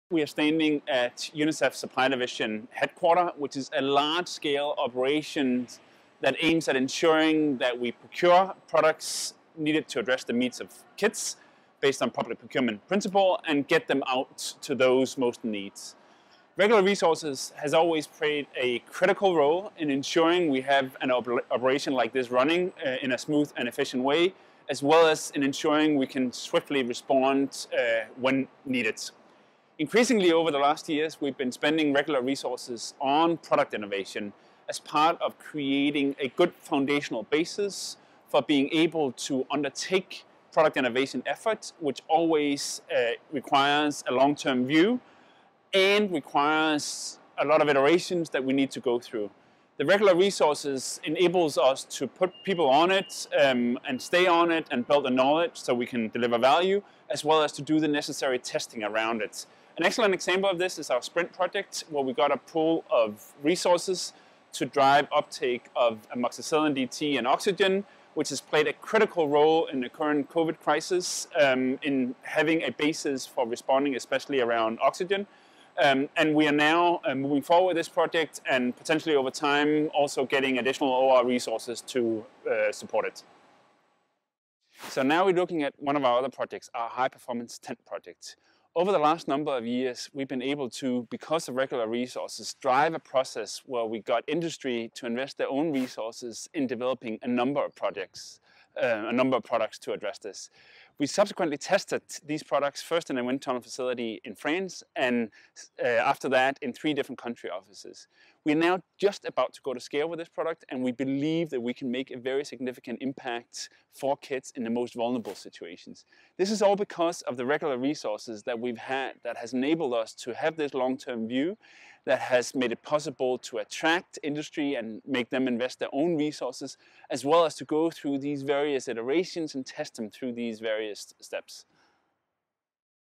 Kristoffer Gandrup-Marino discusses the importance of regular resources for product innovation
UNICEF Regular Resources is essentially funding without restrictions that has, among other things, resulted in the innovation of High Performance Tents. In this clip, Kristoffer Gandrup-Marino, Chief, Product Innovation Centre, UNICEF Supply Division, discusses the importance of Regular Resources for Product Innovation. This flexible funding has helped UNICEF innovate and scale up proven solutions.